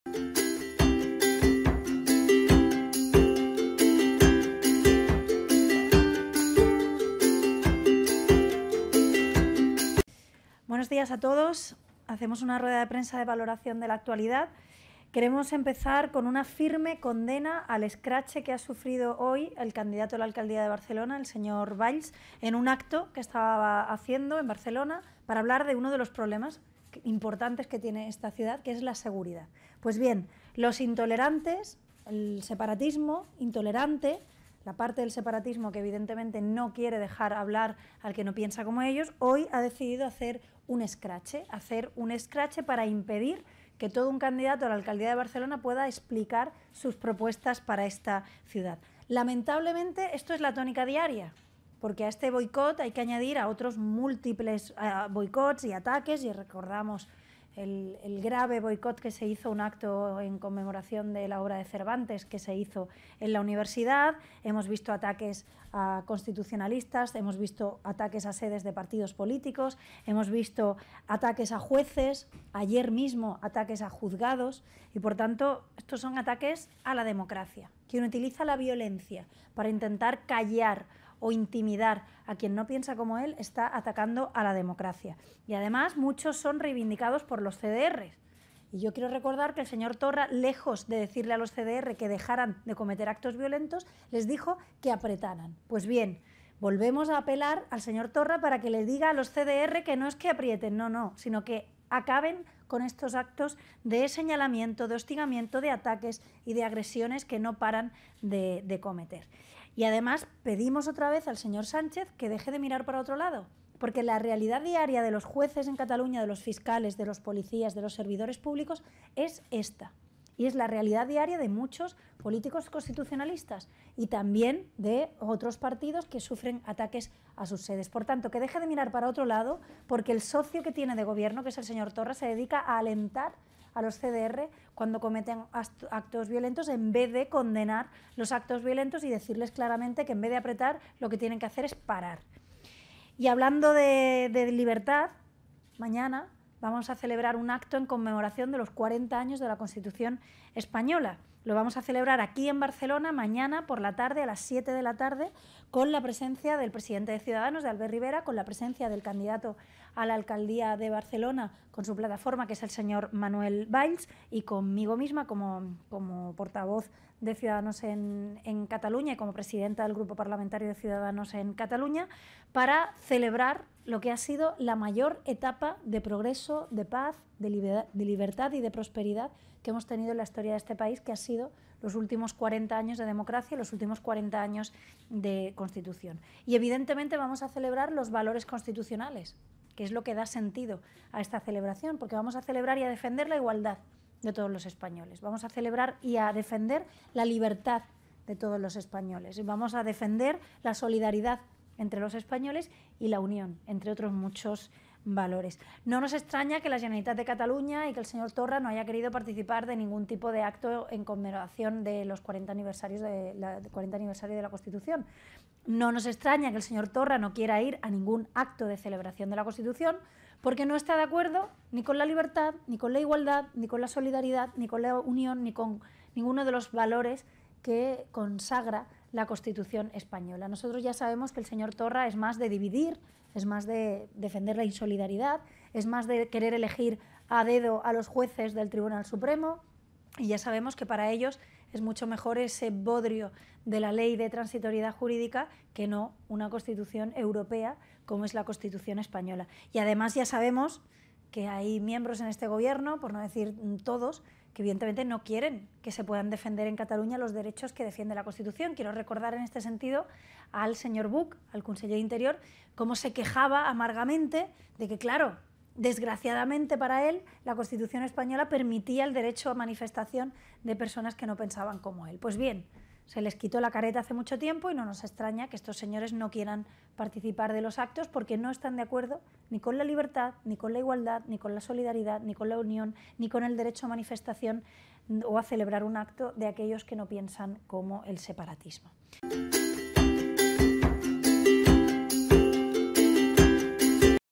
Ines Arrimadas condena el escrache del separatismo intolerante a Manuel Valls
Rueda de prensa en el Parlament de Cataluña 4 de Diciembre de 2018 de Ines Arrimadas condena el escrache del separatismo intolerante a Manuel Valls. Fuente Canal Parlament.
JM TusNoticias. Tu Web de Noticias Actualidad, Política, Sociedad, Ultimas Noticias, Debates. Noticias Independientes y sin ningún tipo de influencias.